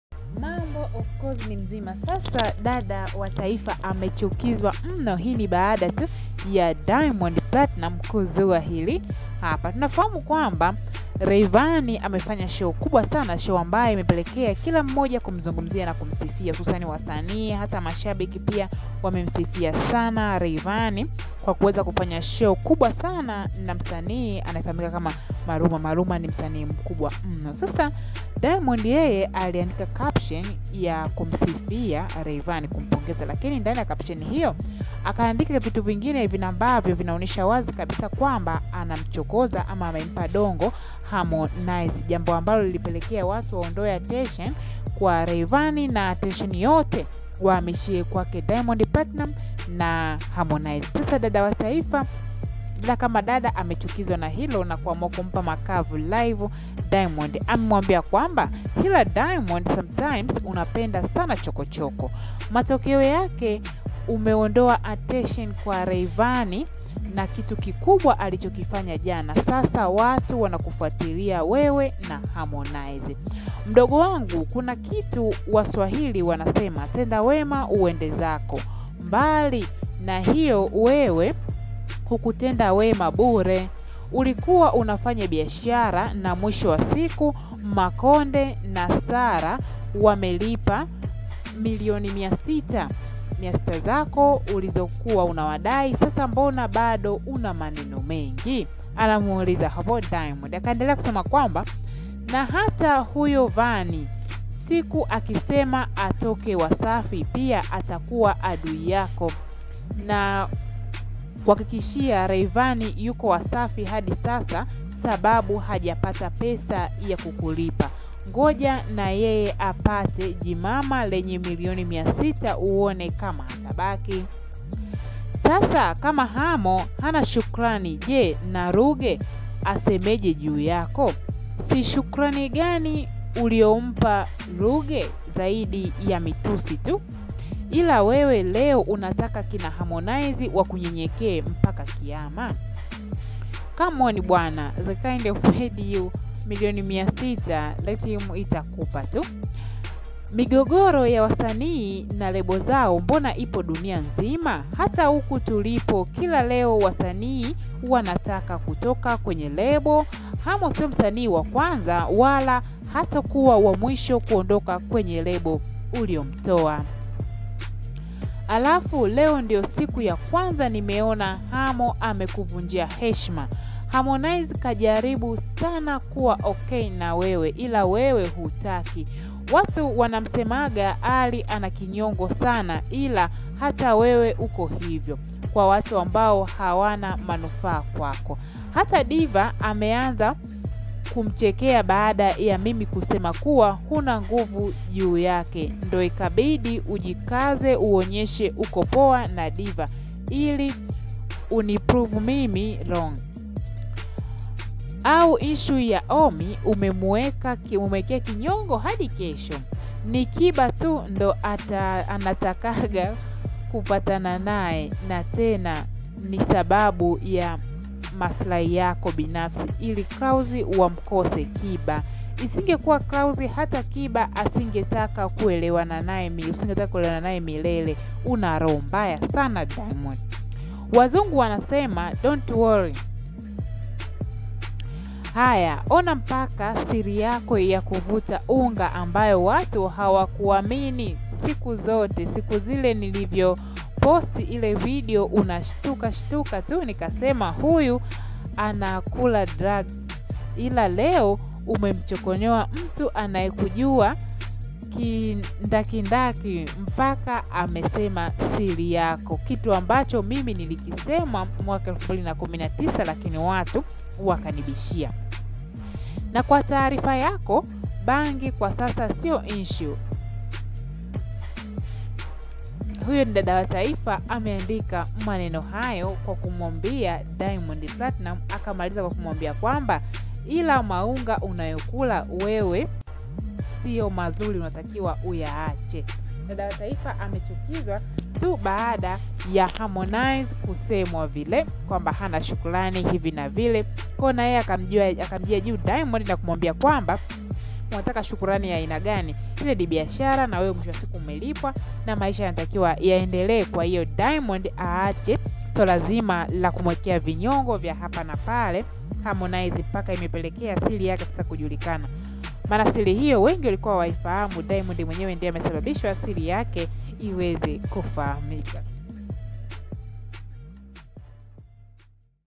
MANGE amvamia DİAMOND acha roho mbaya unapenda sana kiki, unaacha kumsapoti RAYVANY...
MANGE amvamia DİAMOND  acha riho mbya unapenda sana kiki,  unaacha kumsapoti RAYVANY...